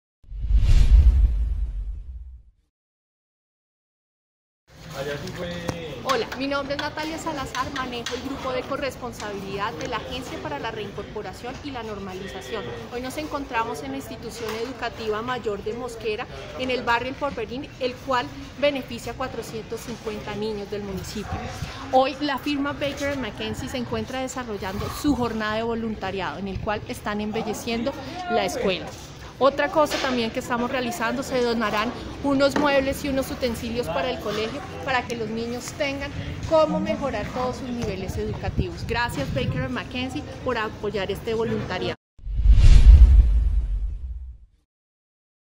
Jornada de voluntariado entre ARN y Baker And Mackenzie en Mosquera
La firma de abogados Baker & McKenzie y la Agencia para la Reincorporación y la Normalización (ARN), lideraron la jornada de voluntariado en la institución educativa Mayor de Mosquera, ubicada en este municipio de Cundinamarca. La intervención permitió que el colegio mejorara su infraestructura, se recuperaran espacios deportivos y se embellecieran importantes zonas de la institución. Cerca de 480 estudiantes se beneficiaron de este voluntariado.